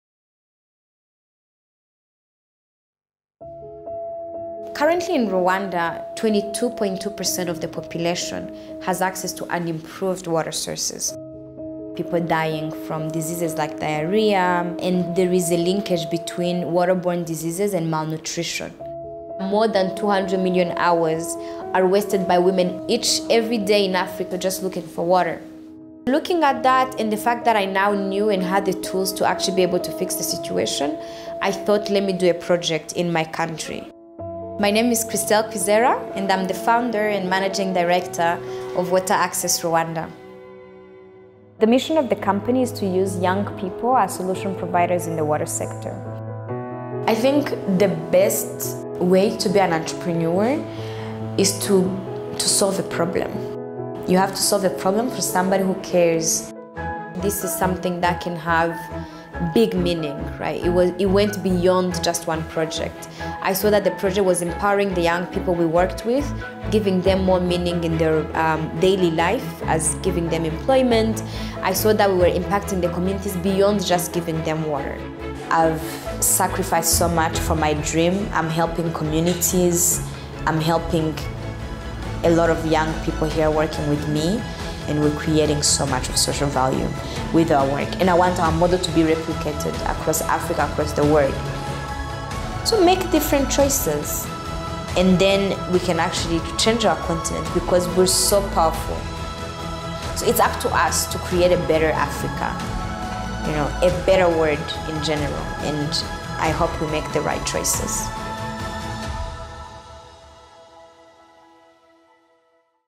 2019 ABH Top10 Christelle Kwizera - Water Access Rwanda
Water Access Rwanda pioneered INUMA™️, a Safe Water Microgrid that reclaims broken boreholes and transforms them into state-of-the-art solar-powered water kiosks and pipelines. The water is sold for $1/1000L and creates off-farm jobs for youth. Currently, Water Access Rwanda employs 68 people and allows 47,612 customers to access water daily across 86 stations.